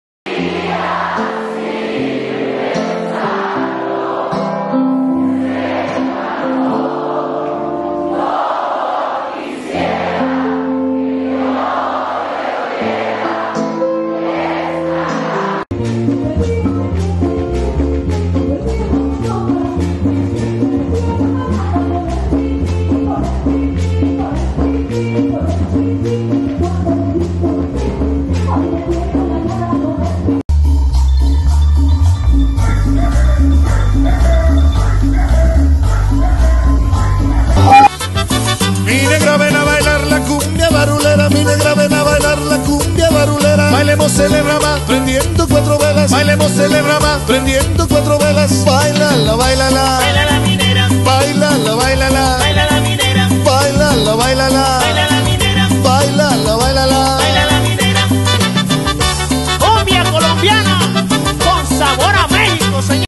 CONTINÚA LA FIESTA EN LA FERIA DE LAS FLORES DE SAN ÁNGEL 2023
Con bombo y platillo se inició la Feria de las Flores San Ángel 2023, más de 12 mil personas disfrutaron en el Parque de la Bombilla de dos conciertos, venta de plantas y flores, comida, así como actividades culturales, en un ambiente totalmente familiar y con saldo blanco.